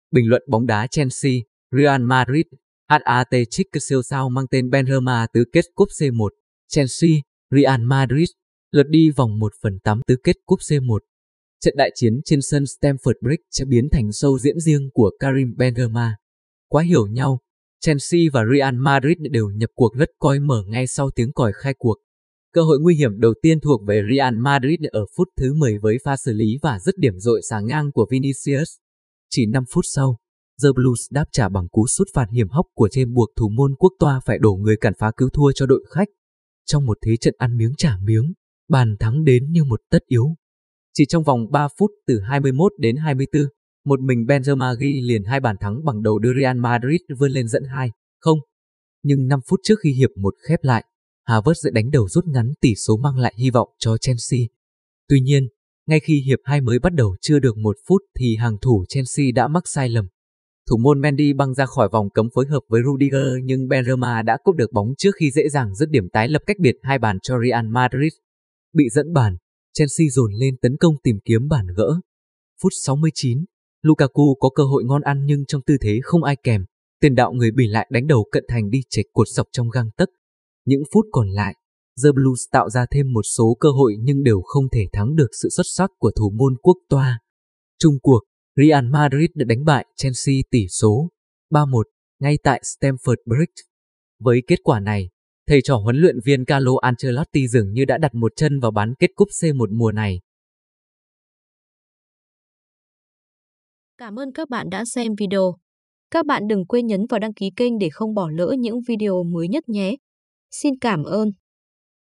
Bình luận bóng đá Chelsea vs Real Madrid sau trận đấu | Hattrick Karim Benzema
Chelsea - Real Madrid, lượt đi vòng 1/8 Tứ kết Cúp C1. Trận đại chiến trên sân Stamford Bridge biến thành show diễn riêng của Karim Benzema.